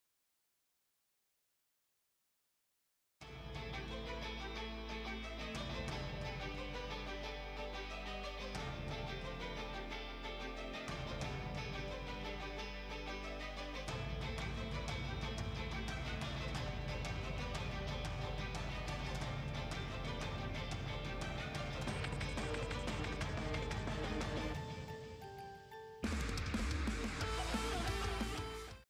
My layout part in "Just a Theory" Hosted
Here is my part in just a theory mega collab which is subject to change based on your guys feedback.
Feel free to say anything about this layout in the comments even if bad as this is one of my very first collabs and even layouts!